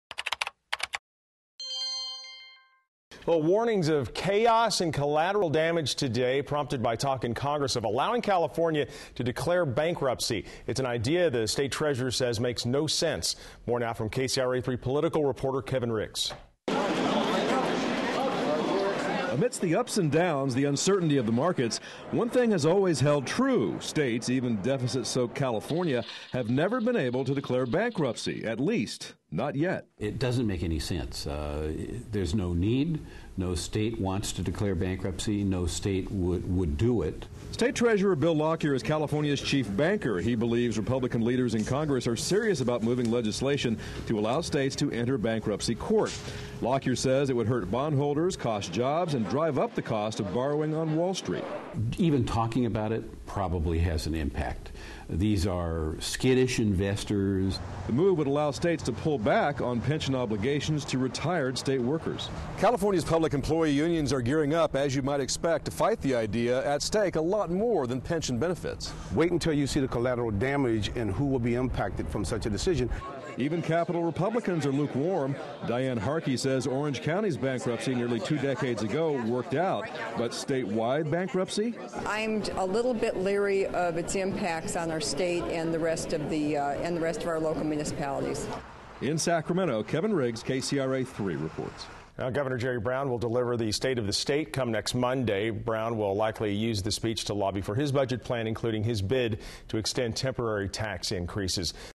Is California Heading For Bankruptcy?
Allowing California to declare bankruptcy "doesn't make any sense," according to State Treasurer Bill Lockyer.